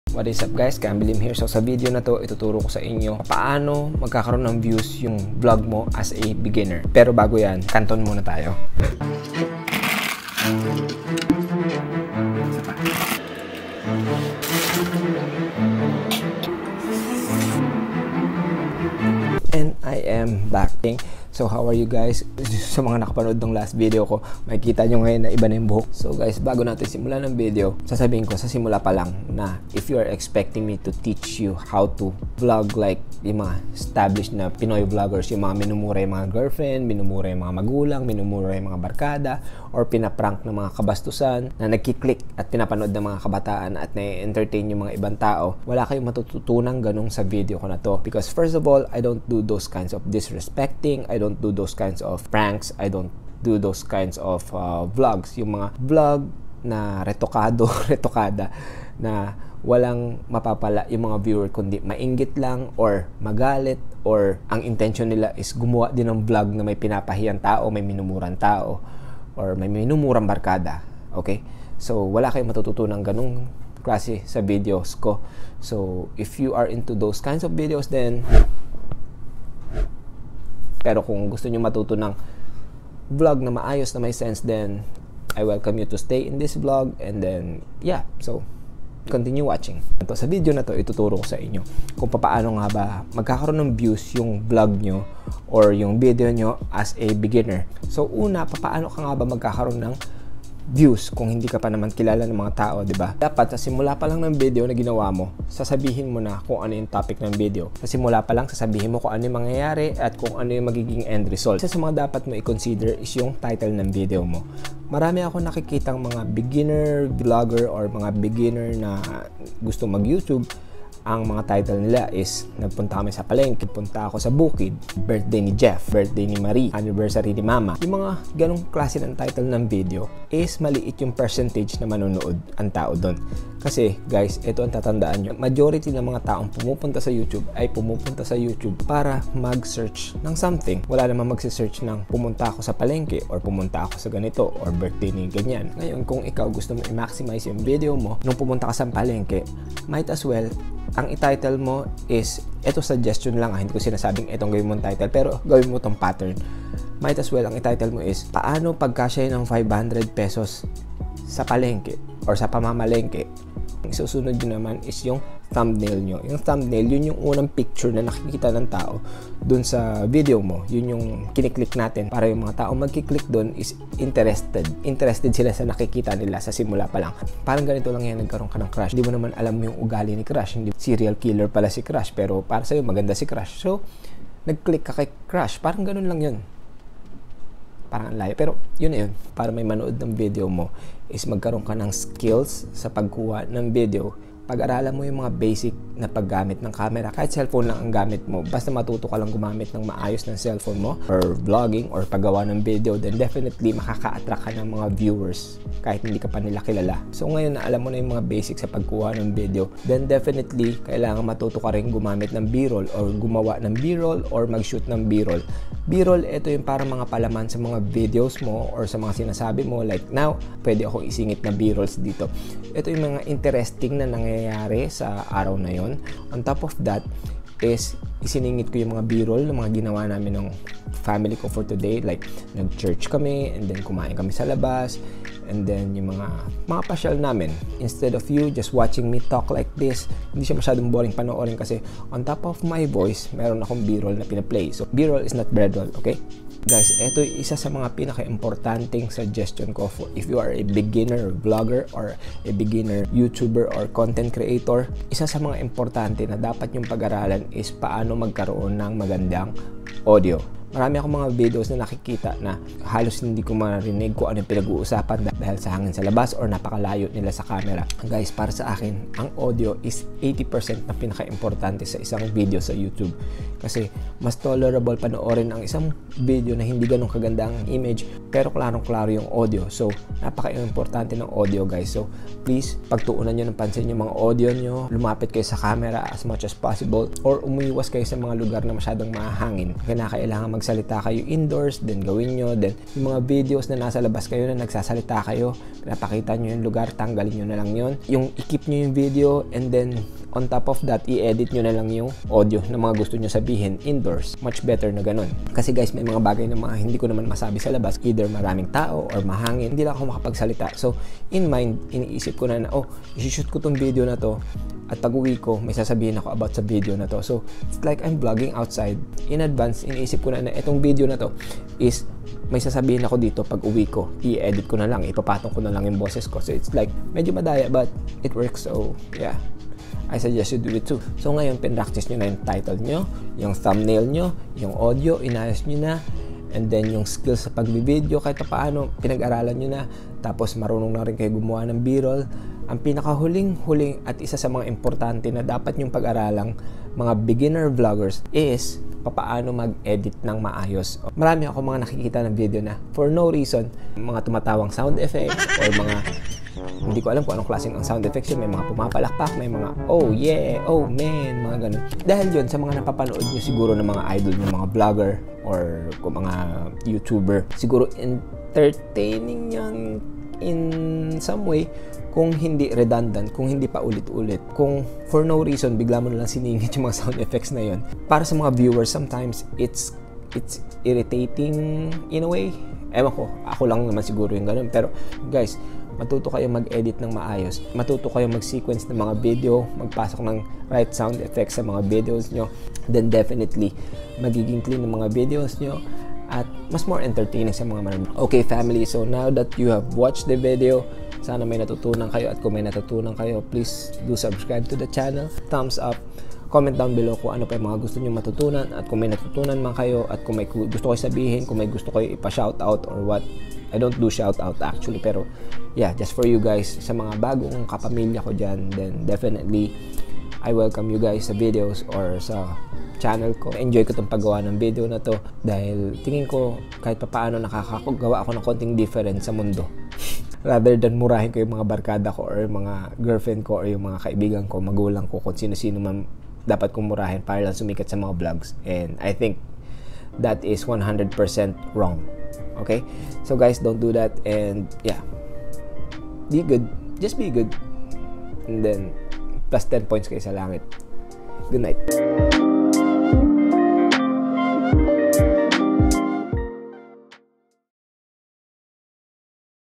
Tips para sa beginners for views.
Sa video na ito I’ll teach you the legit way to get views. Hindi ko kailangan ibenta dignidad mo para lang sumikat sa YouTube. Sa Tamang PARAAN slowly but surely you’ll make it.